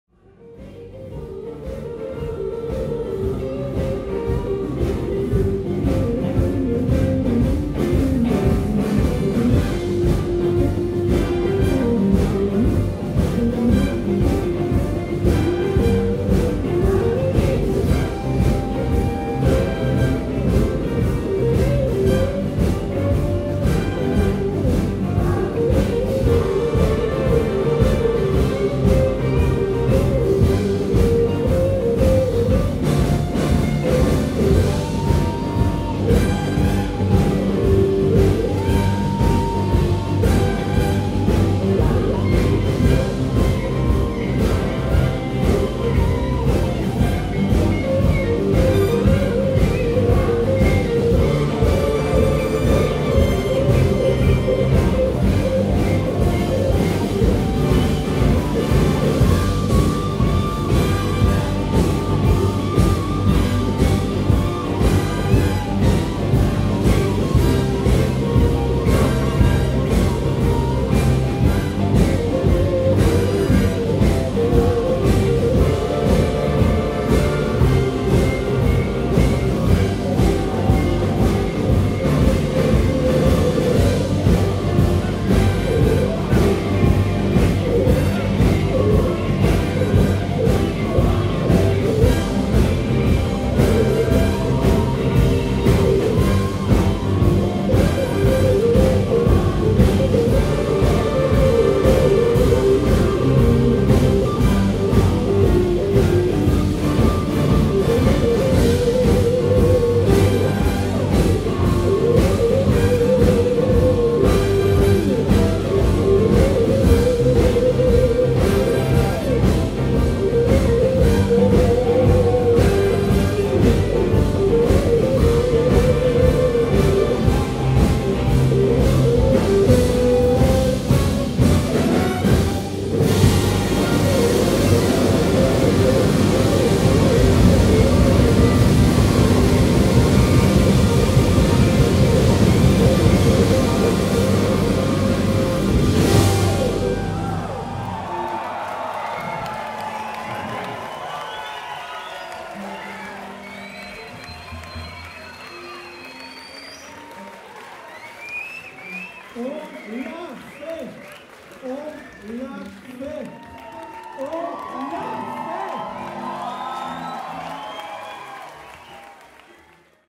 Ton sur Ton Blues Record
Le 21 octobre 2017 Ton sur Ton réalisait un événement FOU: jouer du blues à 288 musiciens dans son bâtiment siège de La Chaux-de-Fonds, en Suisse. Des mois de préparation pour ce défi technique et humain. Le record fut retransmis sur écran géant, et des milliers de spectateurs purent trembler en déambulant au sein de cet orchestre légendaire!